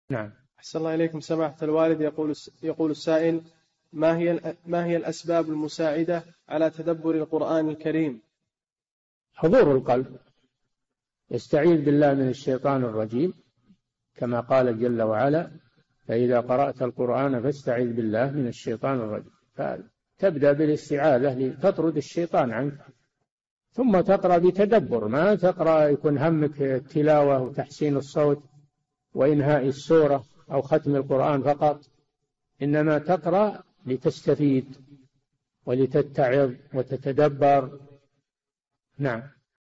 [1555- 4717] ما هي الأمور المعينة على تدبر القرآن؟ - الشيخ صالح الفوزان
[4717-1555] ما هي الأسباب المساعدة على تدبر القرآن الكريم؟
الجَوَابُ: حضور القلب يستعيذ بالله من الشيطان الرجيم كما قال جل وعلا ﴿فَإِذَا قَرَأْتَ الْقُرْآنَ فَاسْتَعِذْ بِاللَّهِ مِنَ الشَّيْطَانِ الرَّجِيمِ﴾ [النحل: 98] فتبدأ بالاستعاذة لتطرد الشيطان عنك، ثم تقرأ بتدبر ما تقرأ ويكون همك التلاوة وتحسين الصوت، وإنهاء السورة أو ختم القرآن فقط، إنما تقرأ لتستفيد، ولتتعظ وتتدبر.
السيرة الذاتية :
للاشتراك في قناة الشيخ صالح الفوزان
لمشاهدة حلقات فتاوى على الهواء كاملة

للاستفسار والإقتراحات والدعم
رئيس الفريق
لمشروع كبار العلماء